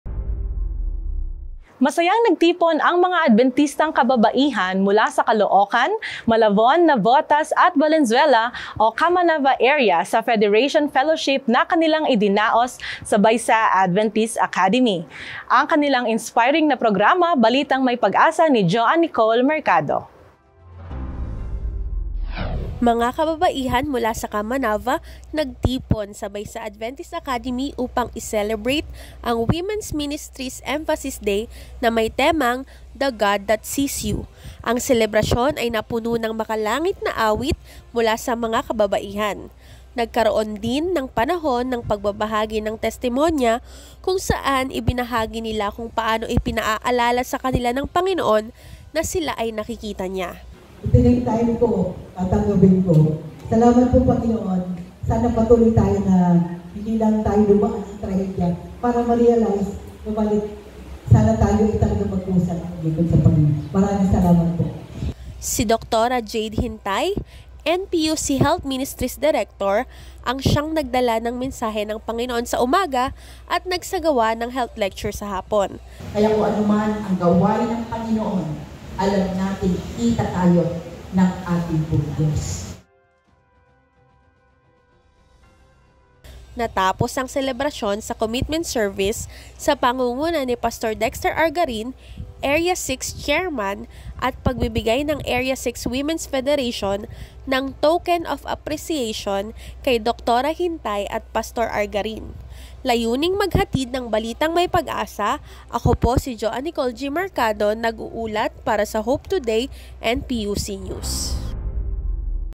Bagong inspirasyon naidulot ng Federation fellowship sa buhay ng CAMANAVA Area Adventist Women
Masayang nagtipon ang mga Adbentistang kababaihan mula sa Caloocan, Malabon, Navotas at Valenzuela o kilala rin bilang CAMANAVA Area, sa Federation fellowship na kanilang idinaos sa Baesa Adventist Academy.
Ang kanilang inspring na progama, balitang may pag-asa ni Joan Nicole Mercado…